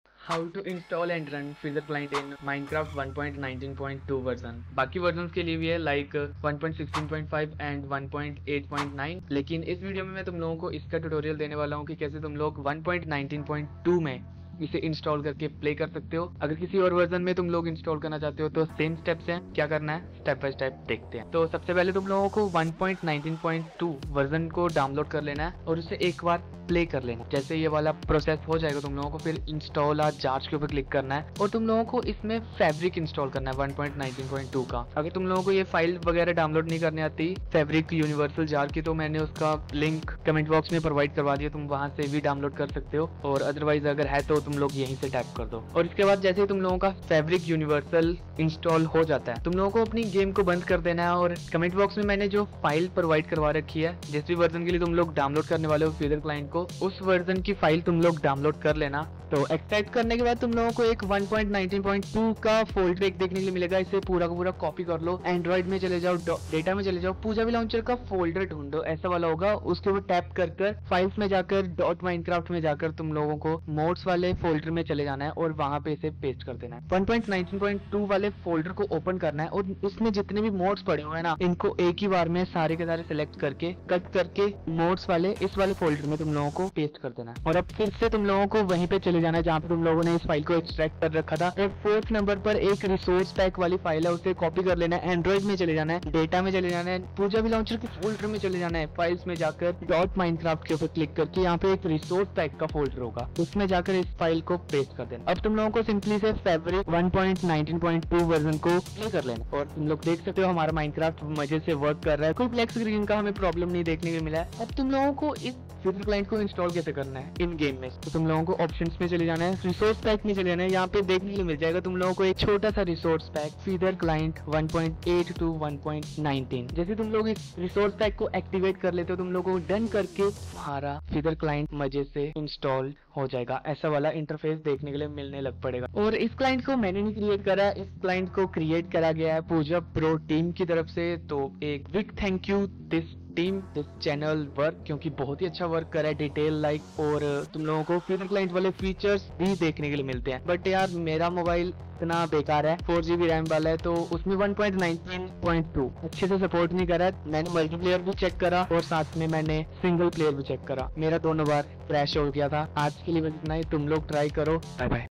How to install and run Feather client in pojavlauncher in simple way (2023)
Hello guys,

In this video, I will show you how to you can use Feather client in your Pojav launcher application without faced any troubles or errors.

• Other platform ✅